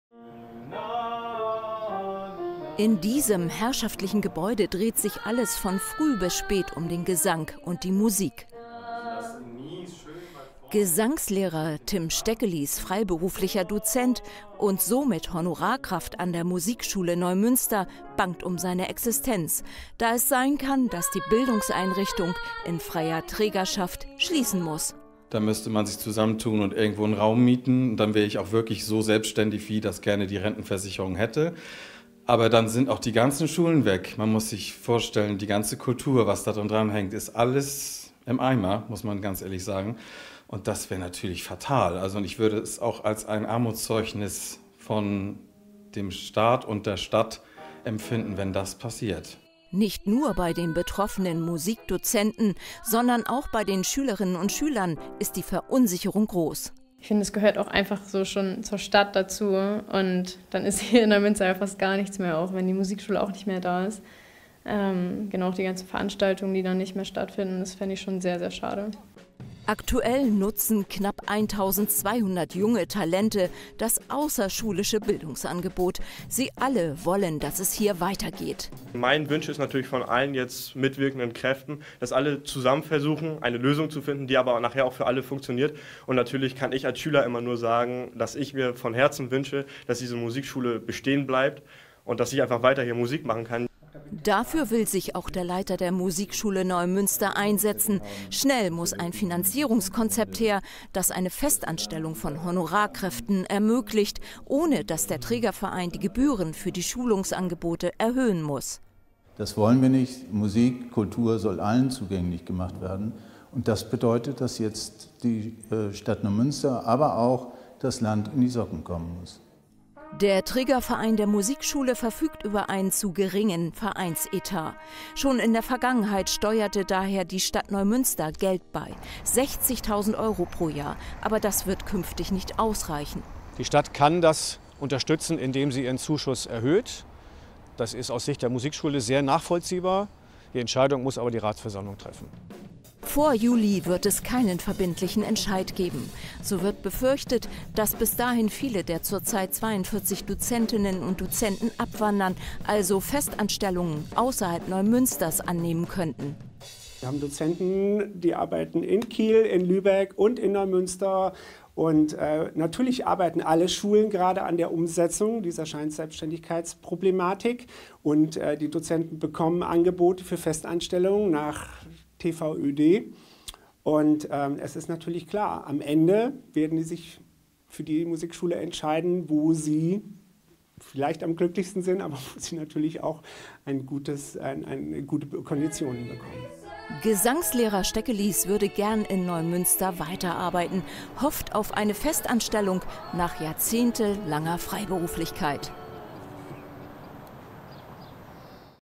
Existenzangst: Musikschulen dürfen keine Honorarkräfte mehr beschäftigen
Laut eines Urteils des Bundessozialgerichts dürfen Musikschulen im Schleswig-Holstein keine Honorarkräfte mehr beschäftigen, sondern müssen alle Dozent:innen fest anstellen. Das ist aber vor allem den Schulen in freier Trägerschaft kaum möglich. Zu gering sind die Etats der Trägervereine. Ein Beispiel: Die Musikschule in Neumünster. Dort unterrichten zurzeit über 40 Honorarkräfte. Sie alle bangen um ihre Existenz und die Musikschüler:innen haben Angst, bald auf ihren Unterricht verzichten zu müssen.

Du hast einen Themenvorschlag für uns?